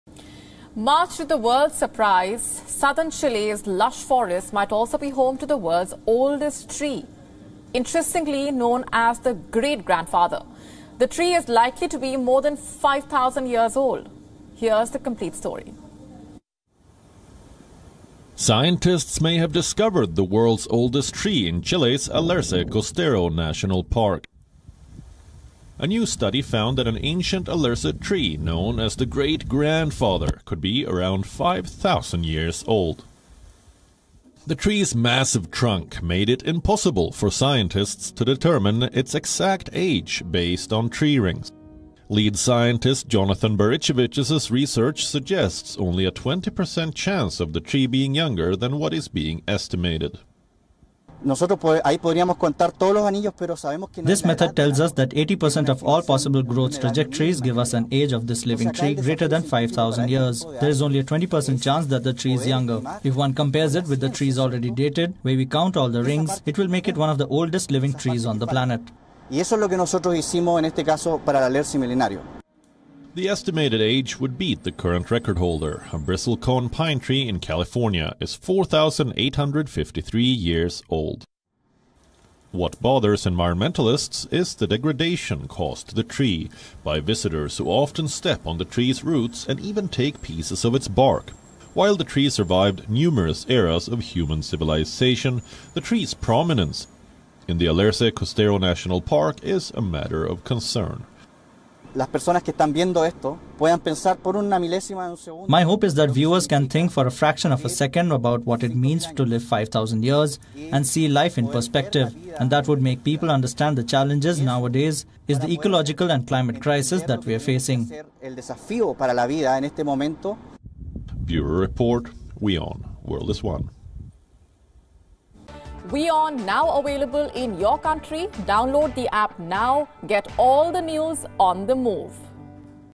Scientists: One of the oldest living trees in Chile | WION Climate Tracker
Much to the world's surprise, Southern Chile's lush forest might also be home to the world's oldest tree interestingly known as the Great Grandfather. The tree is likely to be more than 5000 years old